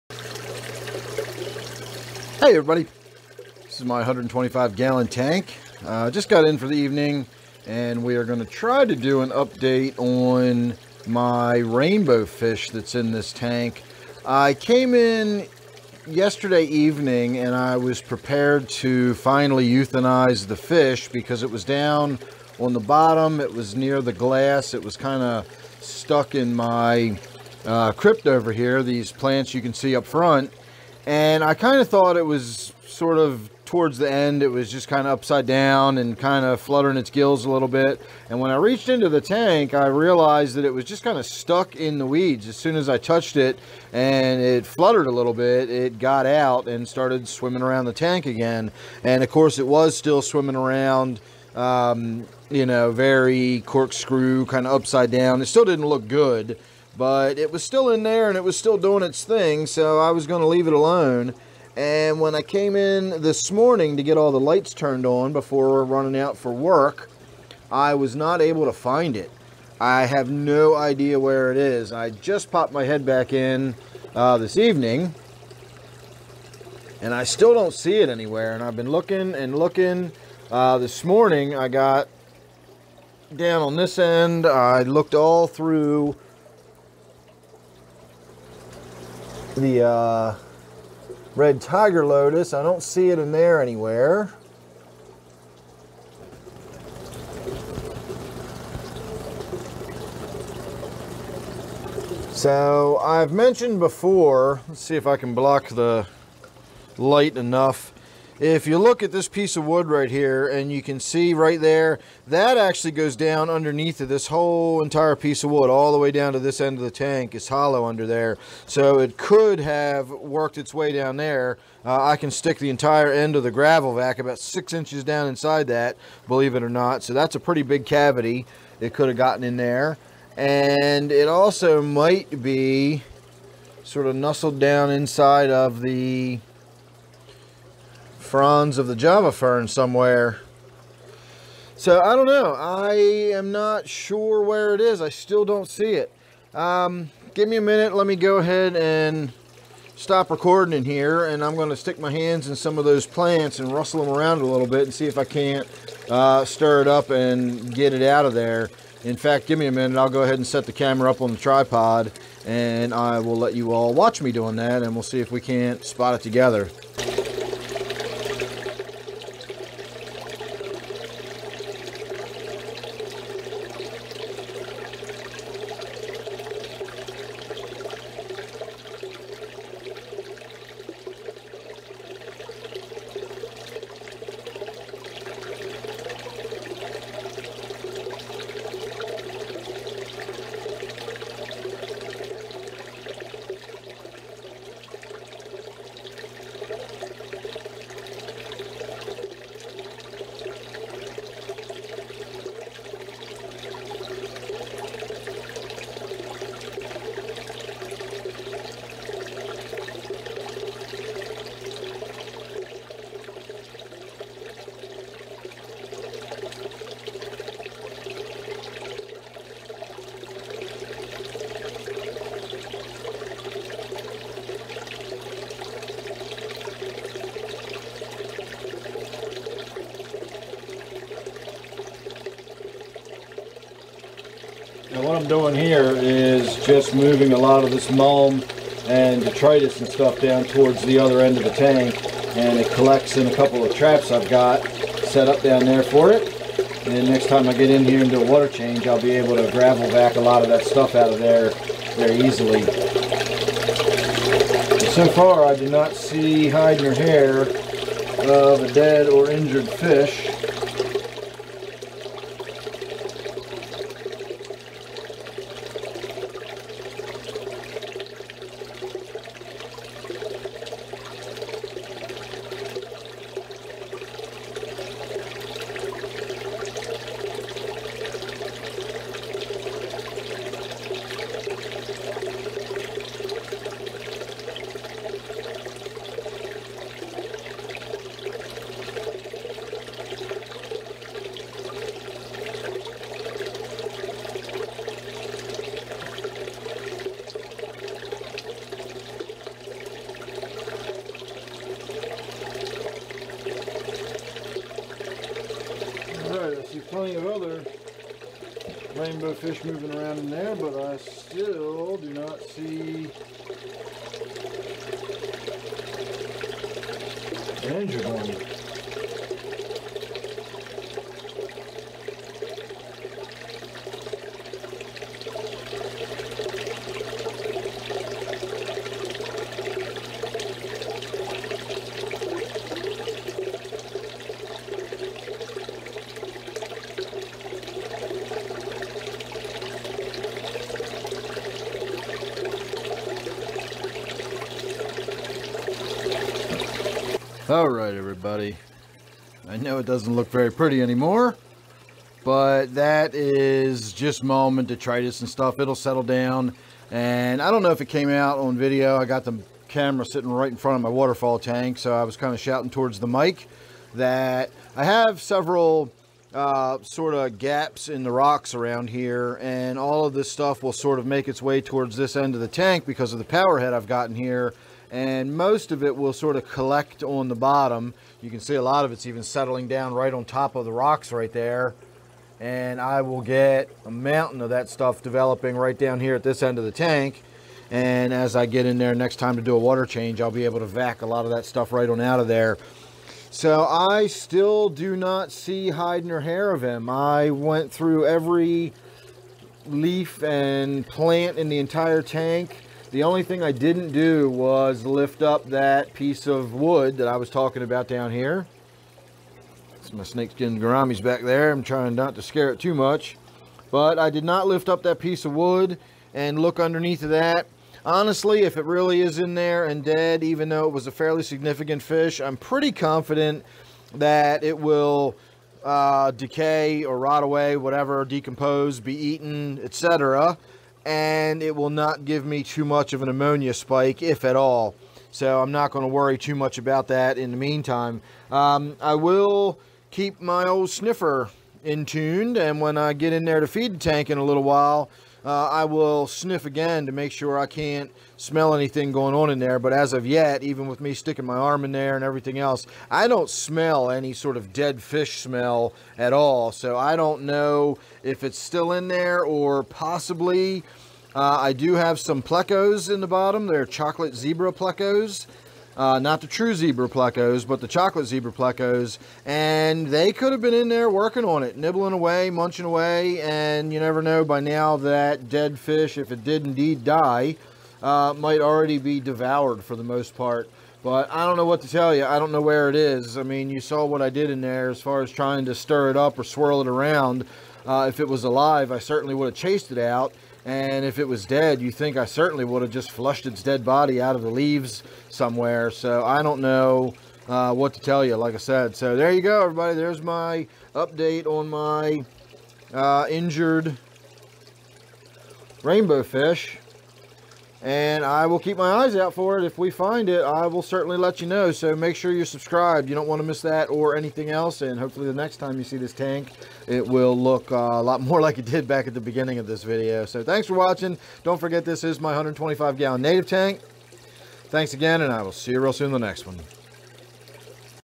Injured Rainbowfish Has Gone Missing
I had a Rainbowfish that was swimming in a bad way, and now I can't find it anywhere in the planted 125 gallon tank.